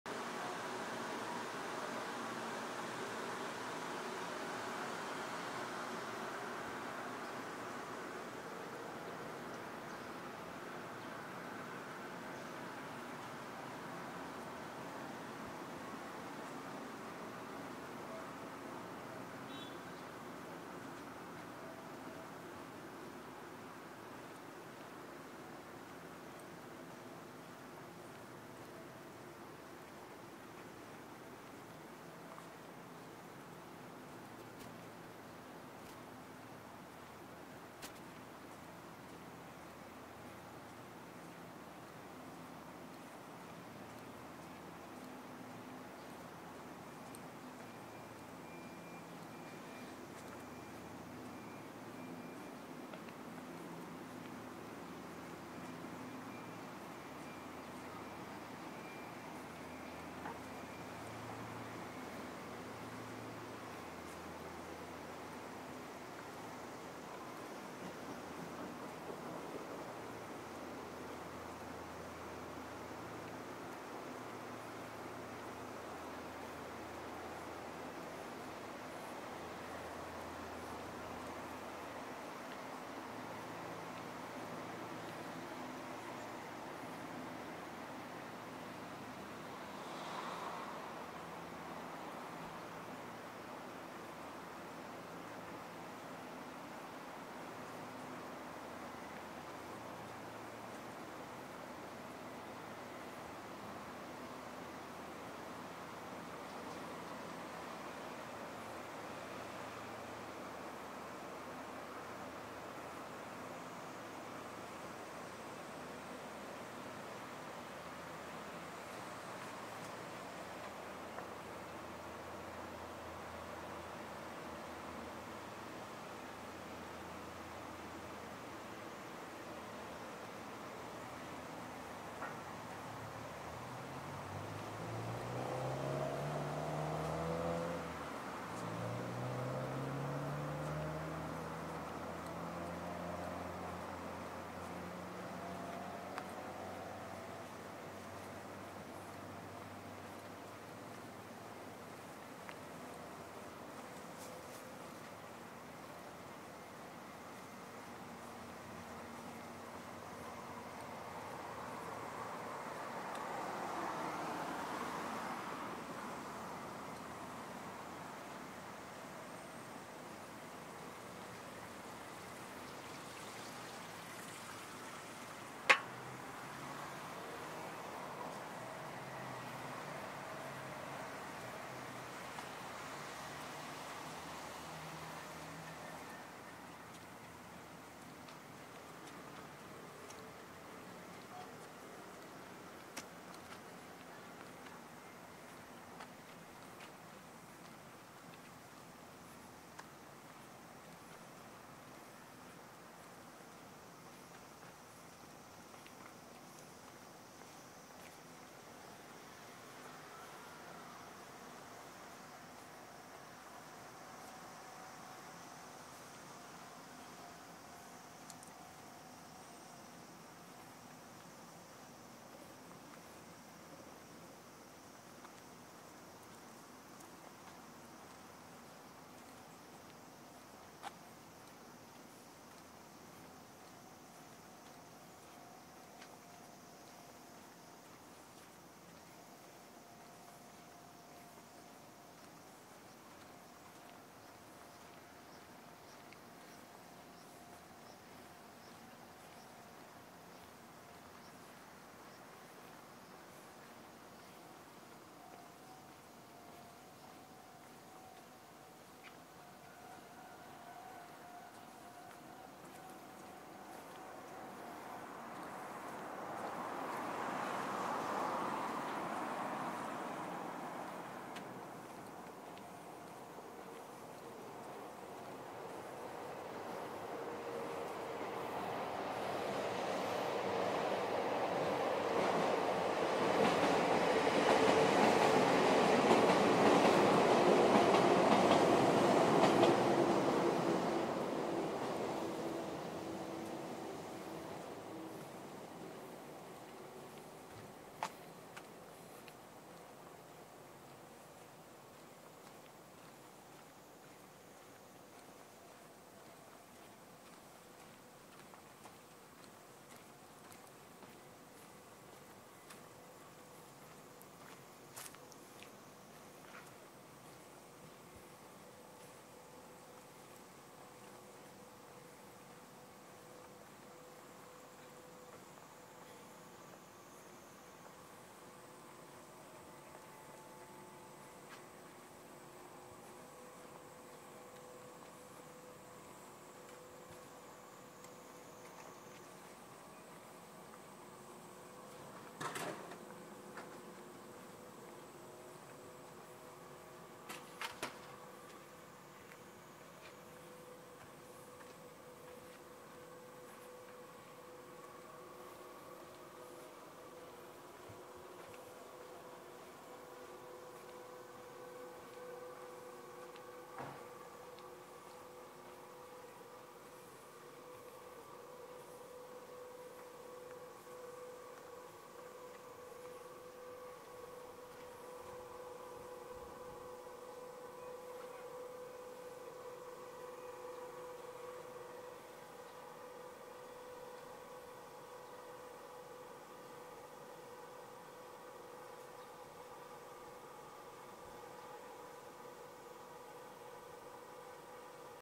神戸市 鈴蘭台駅から旧北区役所周辺を歩き撮り(2023年)
神戸市北区 神戸電鉄 鈴蘭台駅から徒歩5,6分のところにあった旧北区役所の現在の様子を見てきました。
また途中の鈴蘭台プラザも変化がありましたので一緒に見ています。
2023年07月撮影